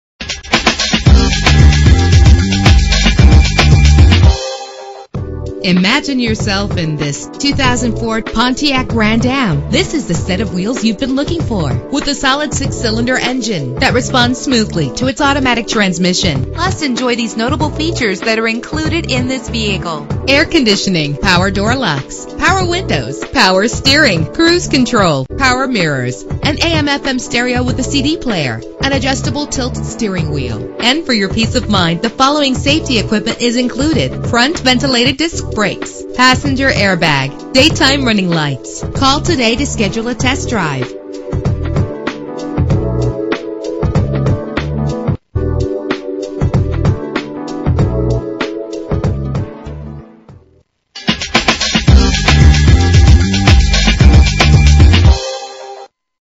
2004 Pontiac Grand Am - Springfield OH
Year: 2004
Make: Pontiac
Model: Grand Am
Trim: SE1
Engine: 3.4 liter 6 cylinder 12 valve
Transmission: 4-Speed Automatic
Color: Black
Mileage: 95745
Address:
1501 Hillcrest Ave